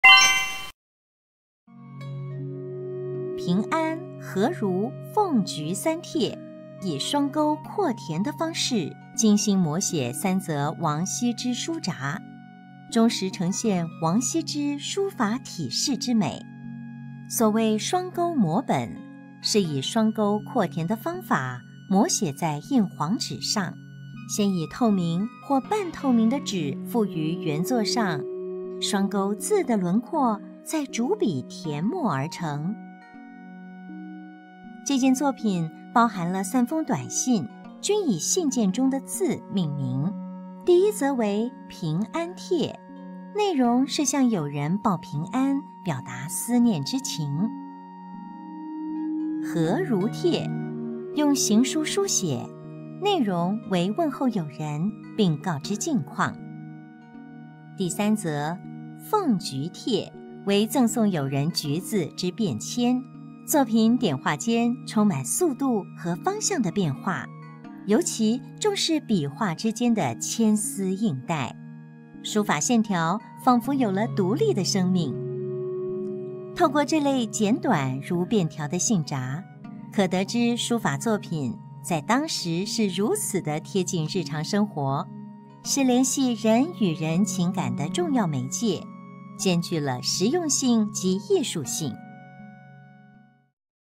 走进台北故宫 · 王羲之《平安何如奉橘帖》
从六世纪南朝时期以来，历代帝王都喜好收藏王羲之等名家的信札，当时将零散的书蹟裱背成一定长度的手卷，后来辗转流传，遭割裂重装，已不复原貌。这卷书蹟是用双钩廓填的方式，复制了三件王羲之短札。《平安帖》行书兼草书，信中提到的“脩载”，是王羲之的堂兄弟。《何如帖》行书，问候对方并告知近况。《奉橘帖》行书，赠送友人橘子并附上此信。